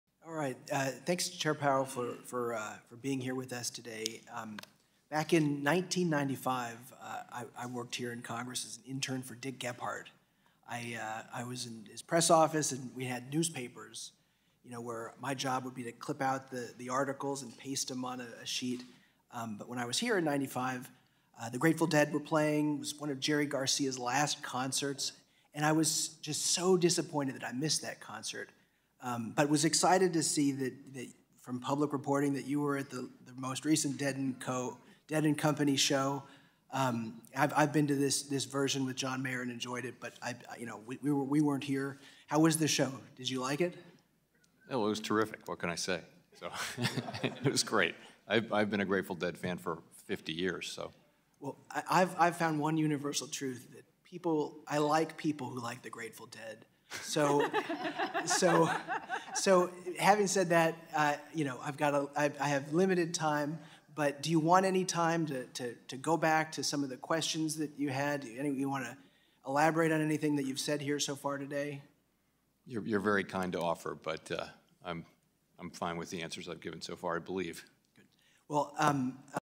Deadhead Jerome Powell
Rep. Wiley Nickel (D-NC) asks about recent Dead & Company concert: "How was this show? Did you like it?"

Fed Chair Powell: "It was terrific. What can I say. It was great. I've been a Grateful Dead fan for 50 years."